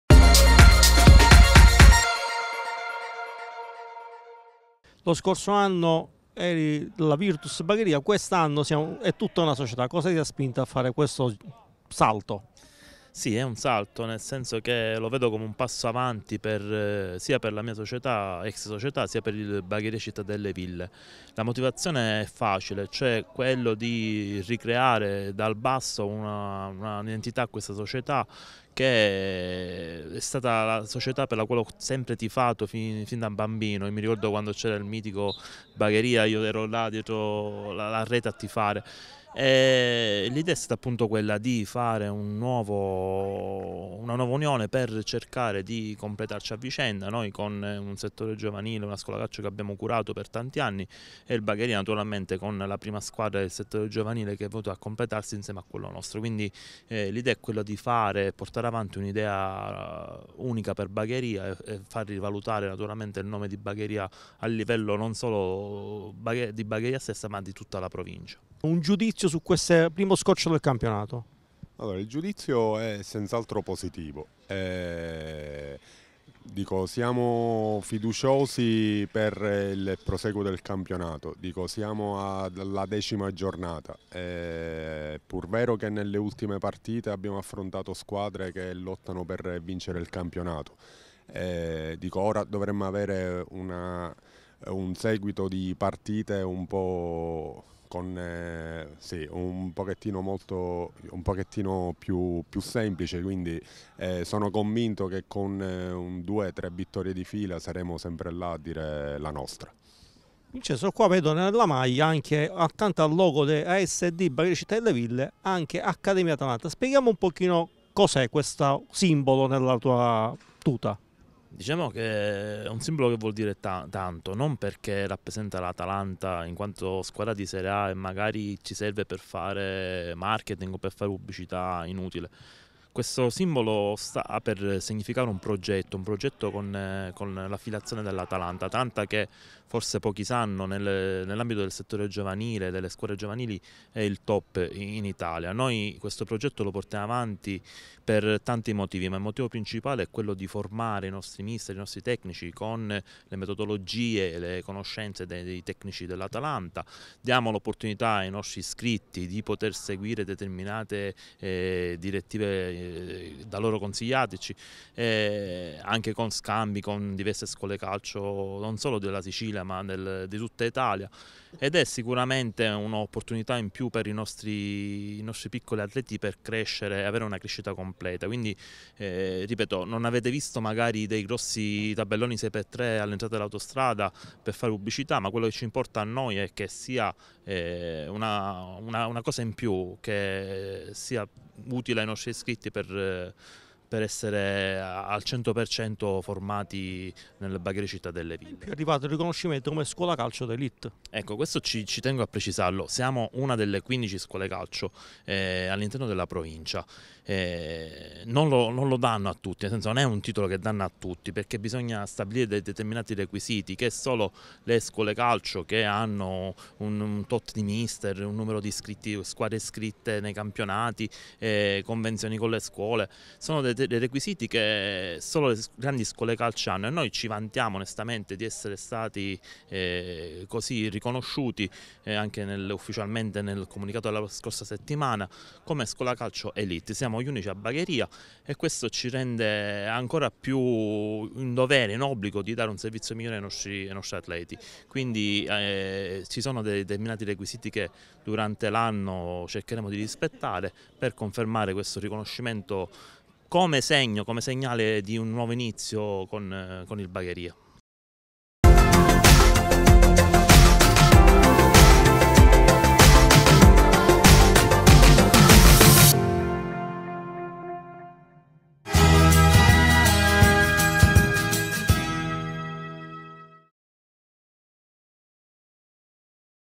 Bagheria Calcio interviste a Vincenzo Bellante e Massimo Balli
Ferve l’attività all’interno del settore giovanile del Bagheria Città delle Ville, da quest’anno affiliata all’Atalanta, abbiamo fatto il punto della situazione con il responsabile Vincenzo Bellante e con il dirigente accompagnatore degli allievi regionali Massimo Balli ascoltiamo cosa hanno dichiarato ai nostri microfoni.